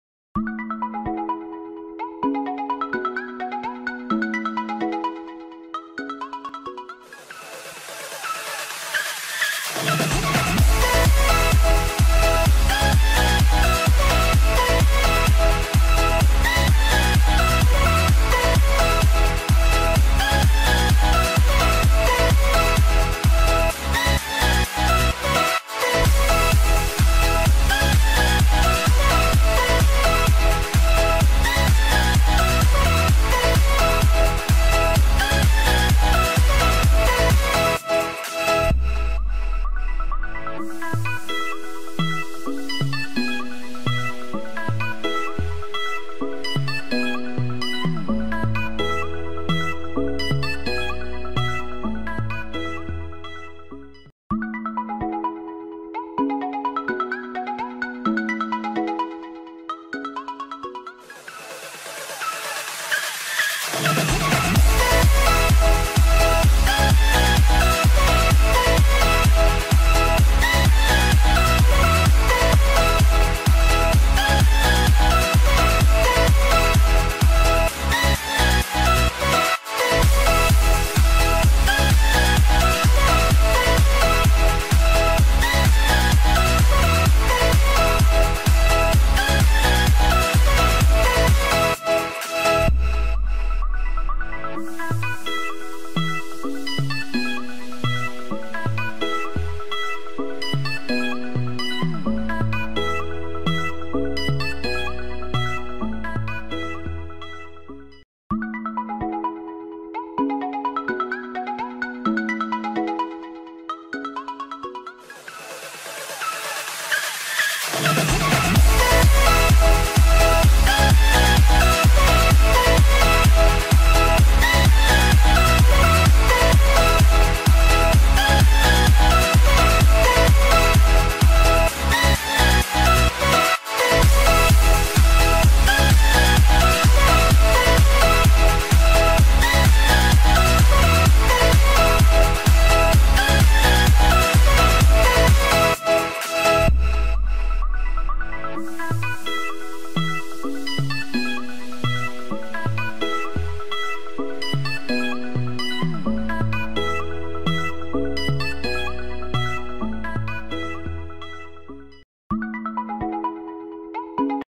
Iball slide elan 4G-2 quick unboxing || high end tablet with massive battery
Features:-
5MP primary camera with LED flash and 2MP front facing camera25.654 centimeters (10.1-inch) IPS HD full multi touch capacitive touchscreen with 1280 x 800 pixels resolution and 149.45 ppi pixel densityAndroid v6.0 Marshmallow  operating system with 1.3GHz ARM Cortex A53 64-bit quad core processor, Mali T720 GPU, 2GB RAM, 16GB internal memory expandable up to 32GB and single SIM7000mAH lithium-polymer battery1 year manufacturer warranty for device and 6 months manufacturer warranty for in-box accessories including batteries from the date of purchase